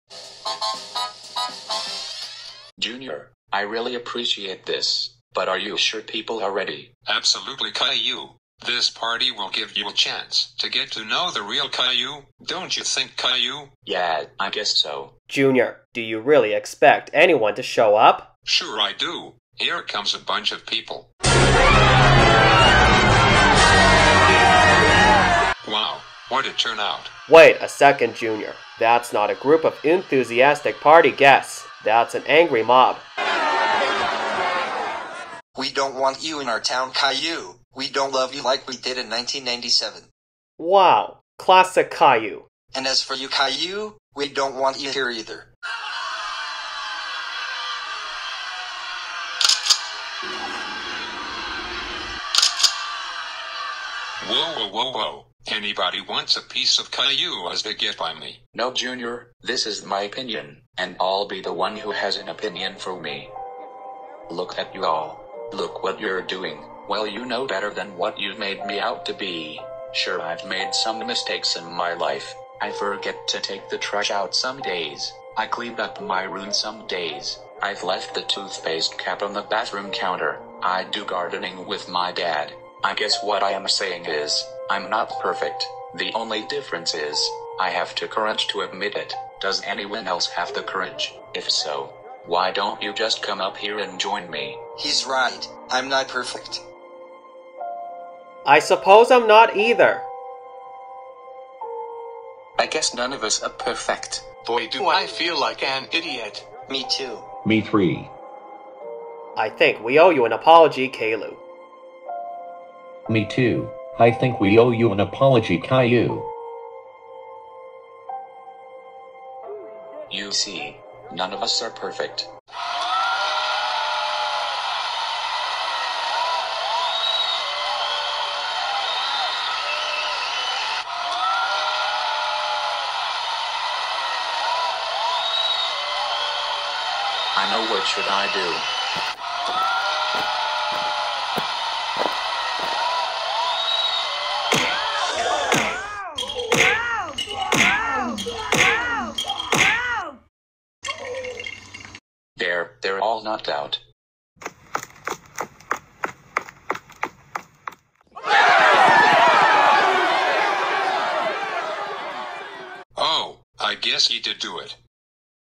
Junior and his family set up for the party for Caillou and the angry mob gets Caillou
Junior brings Caillou to San Diego and Gets Grounded